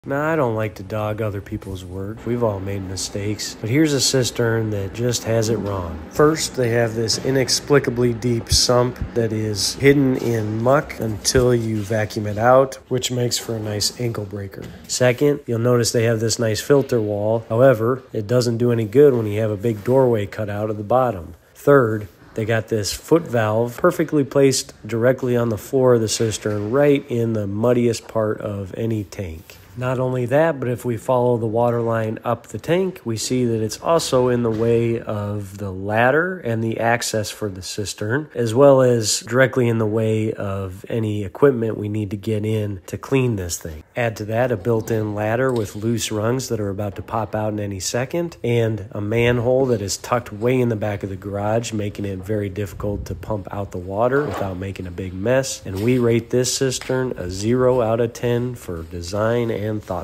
MISTAKES in rain harvesting cistern! Major design flaws found in 1970s concrete cistern
This cistern is the result of poor pre-planning and design. This is a 40,000 gal underground concrete cistern built in the 1970s.